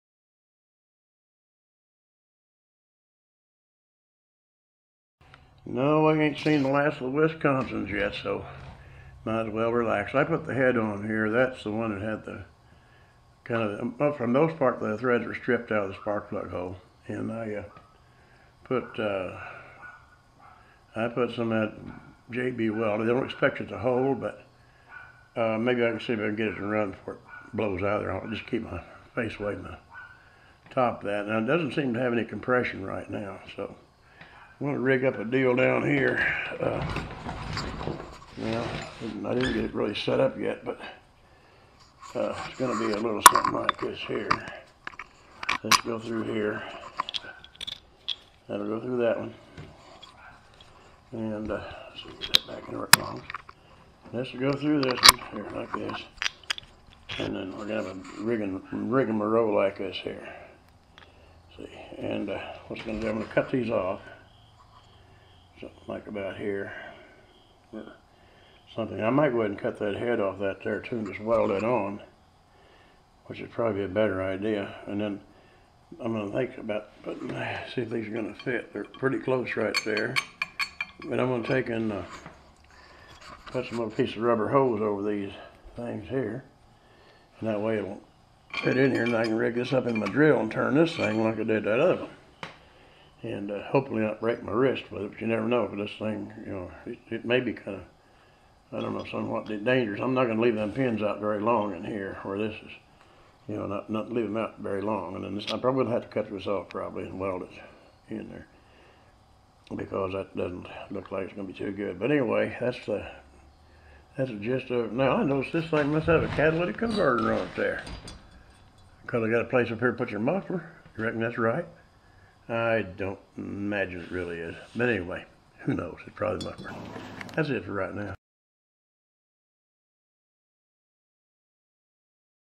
Riggin up an electric start on this old Wisconsin engine
this one has real low compression, the valves seem ok and are adjusted right and the rod's not broke, so it might be the rings, there's not a lot of play in the piston side do side so maybe if I can get it to turn over faster for a while, maybe it will fire off. we shall see in a later video.